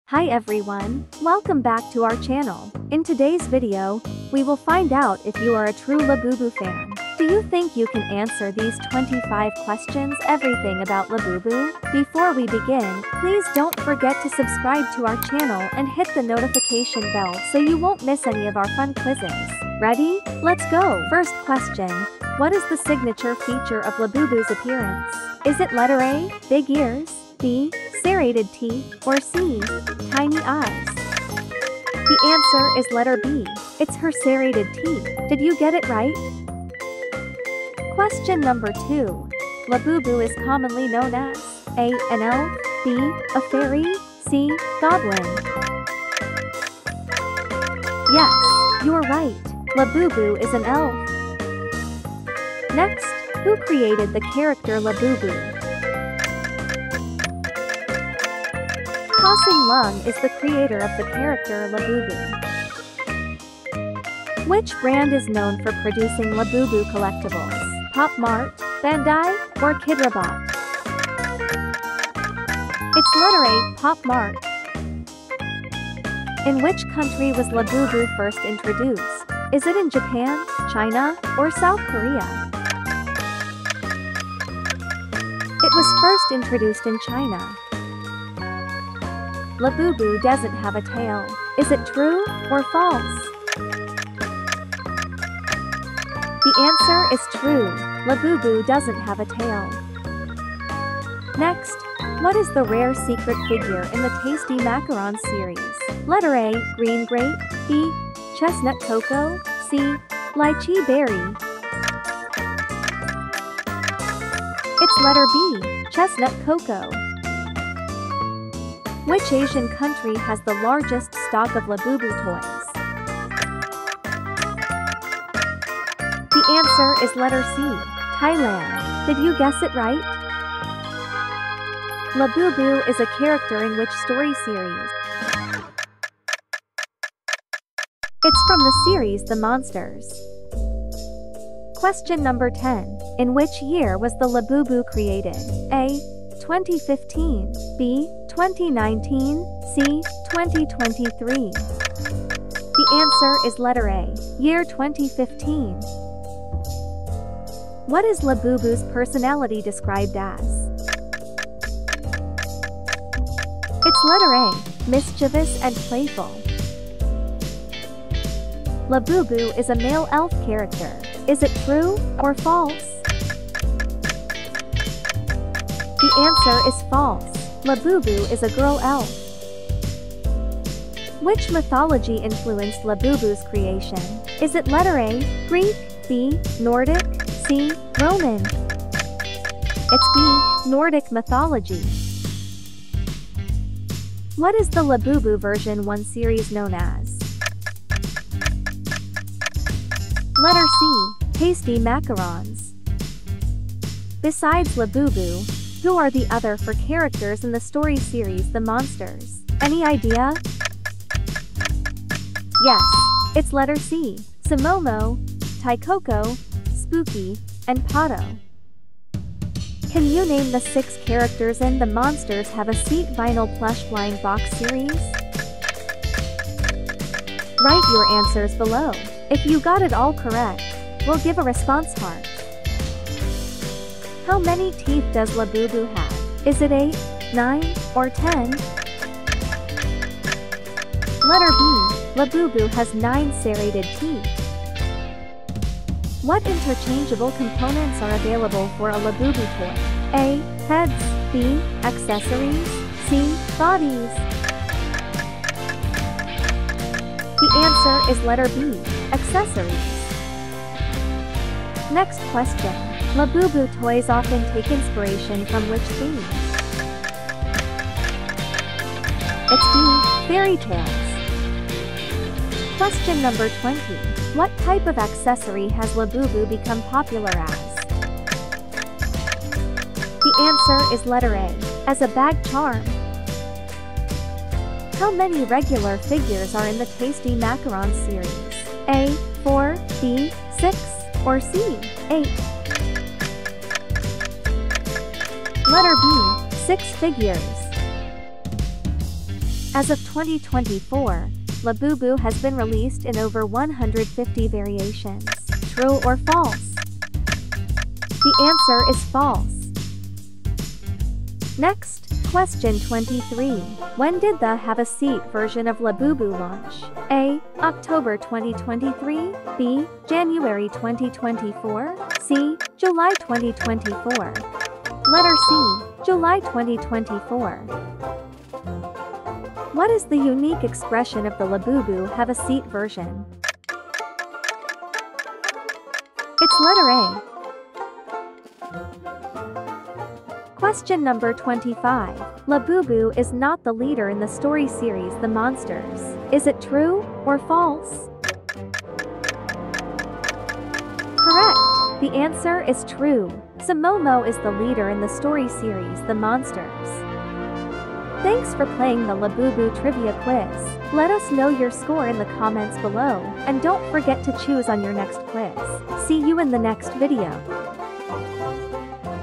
Labubu Lovers Unite! Can You Ace This Labubu Trivia Challenge?!
Ready for some Labubu fun? Can you ace this Labubu trivia challenge?
Take this Labubu Trivia Quiz and challenge your general knowledge about Labubu! We’ve got questions on Labubu collections, appearances, and quirky facts. Don’t miss out – find out if you’re a Labubu expert!